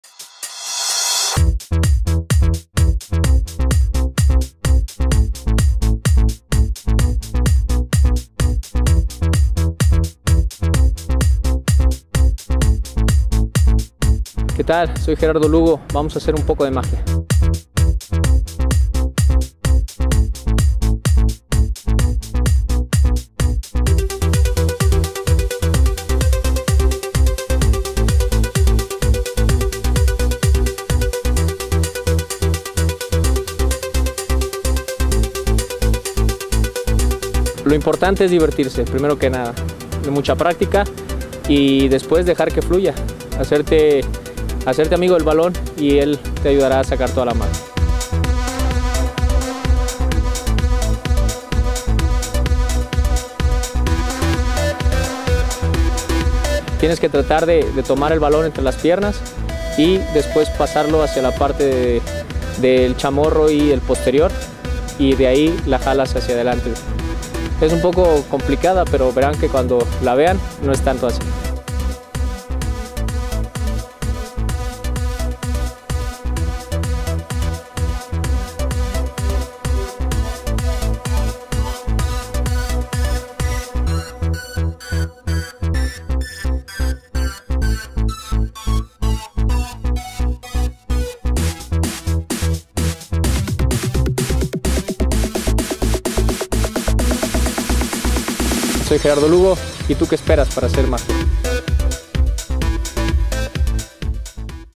LA MAGIA DE GERARDO LUGO
Gerard Lugo nos enseña un truco de Magia. Diviértete y practica las técnicas de tus jugadores favoritos.